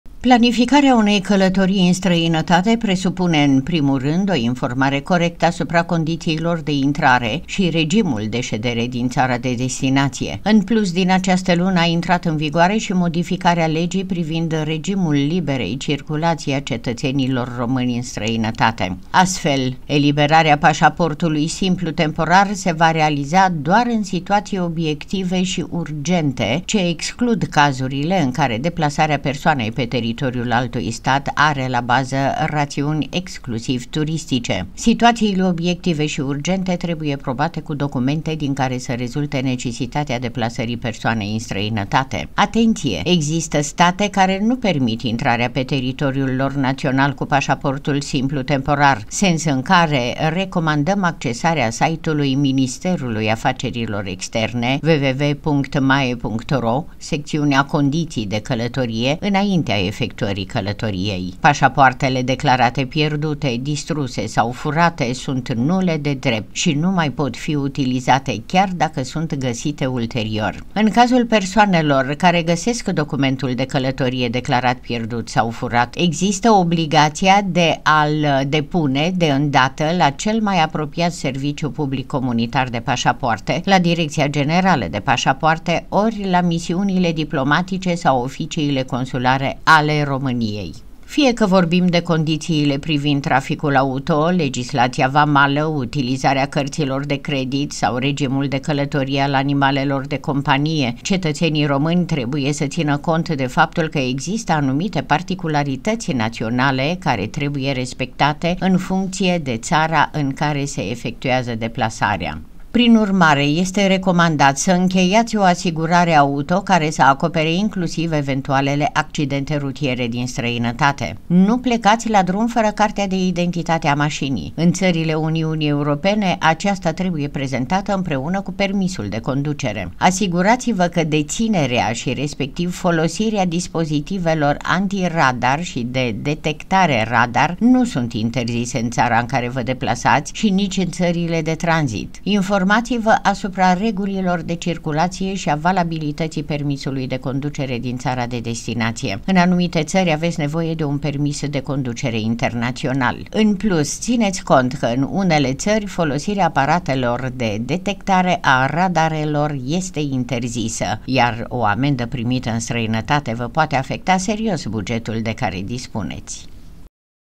INFORMEAZĂ-TE!, PENTRU O VACANȚĂ FĂRĂ GRIJI
DIN ACEASTĂ LUNĂ AU INTRAT ÎN VIGOARE NOI MODIFICĂRI LEGISLATIVE ÎN SISTEMUL DE PAȘAPOARTE